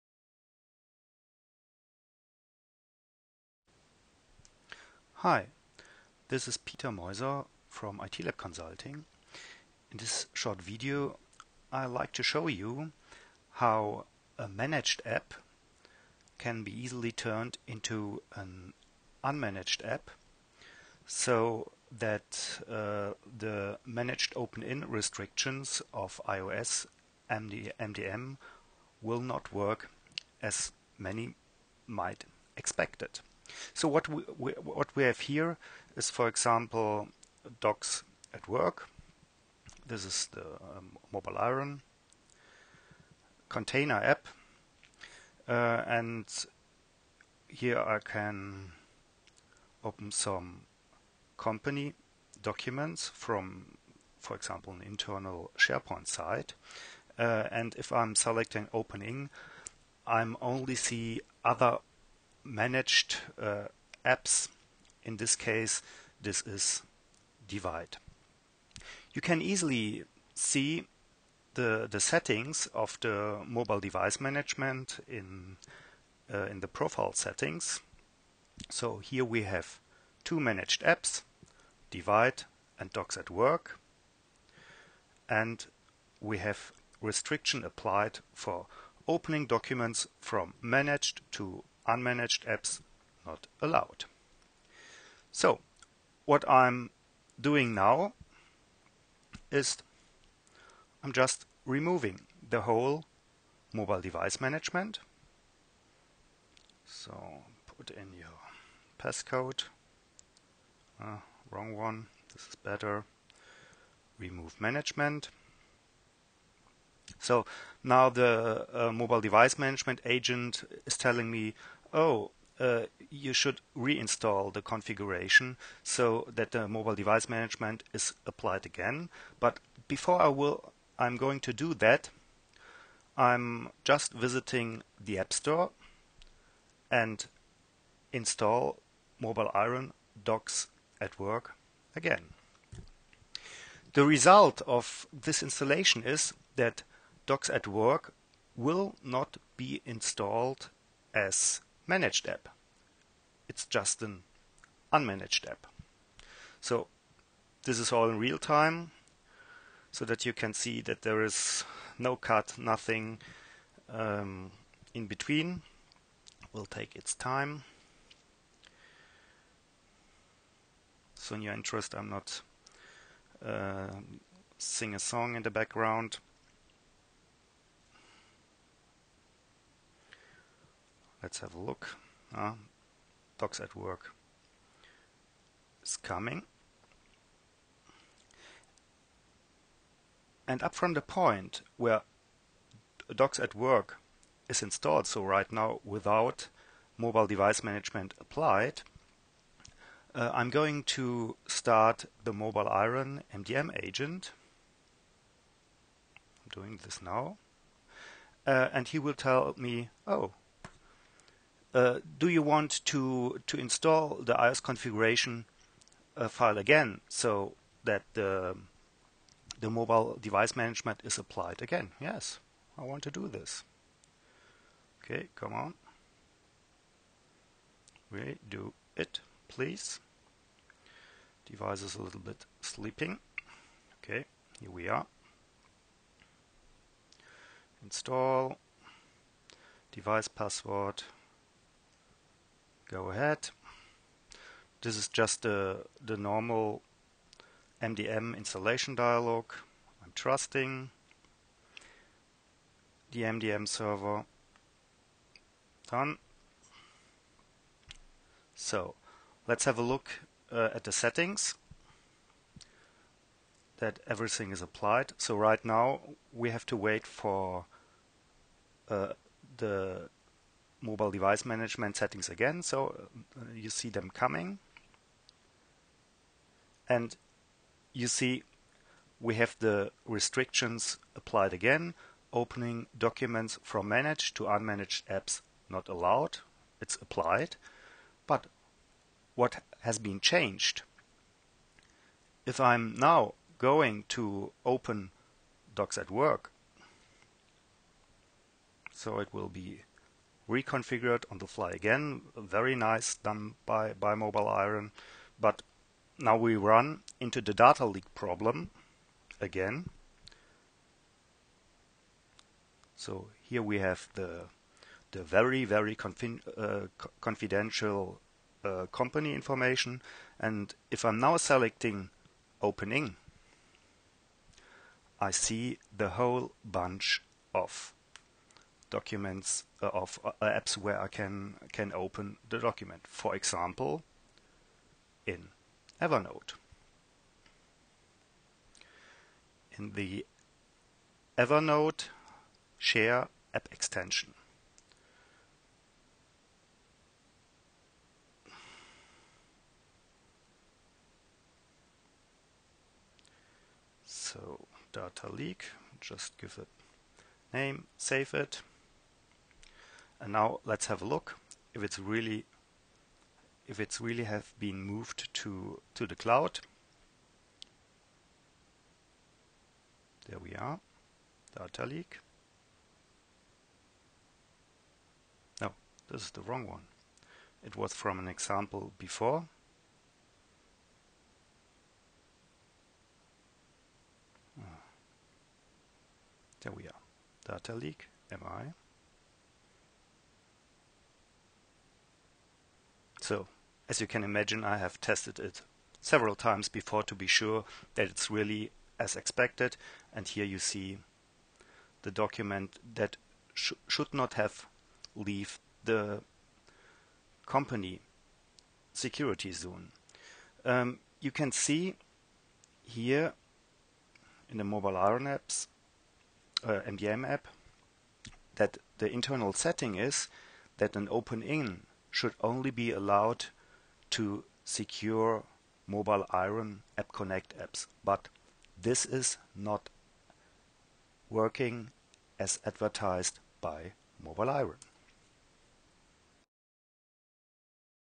MobileIron AppConnect data leak example with Docs@Work
This video shows the data leak issue of MobileIron Docs@Work (as example for other AppConnect apps, too) in combination with iOS 8 app extensions. This issue exists, even if MobileIron's questionable workaround to deploy the app as "managed app" using iOS MDM is applied.

When is MobileIron just going to fix this annoying issue of their "secure" iOS apps?